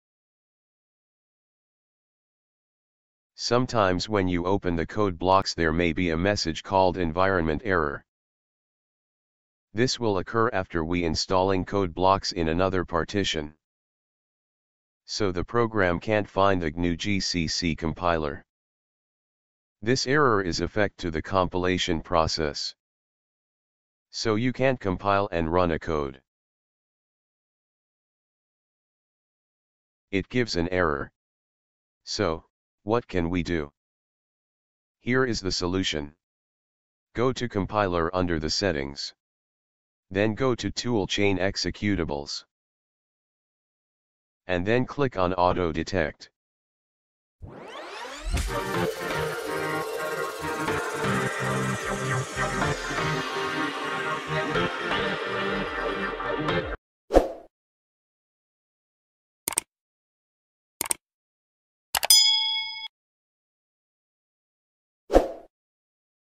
How to solve Environment error in Codeblocks
Today we will discuss about the 'Environment error ' in Codeblocks. This error mainly occurred when we trying to install Codeblocks in an another partition. So let's solve it..

If you like please help me to grow up...